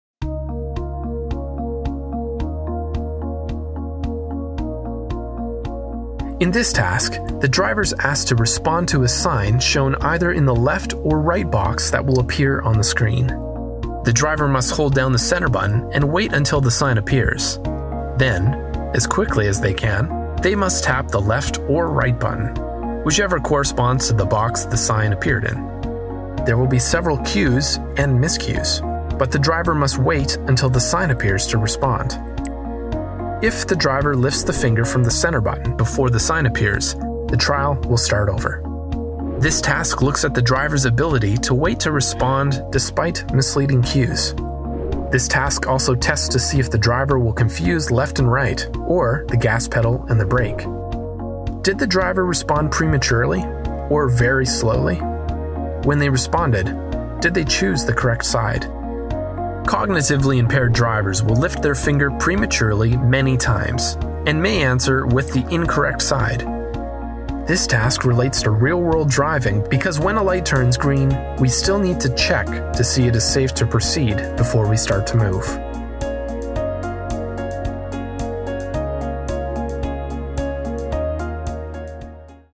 Speed of Attention Shifting
Speed of Attention Shifting helps to determine fitness for driving.

Talk with an expert: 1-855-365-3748, ext. 2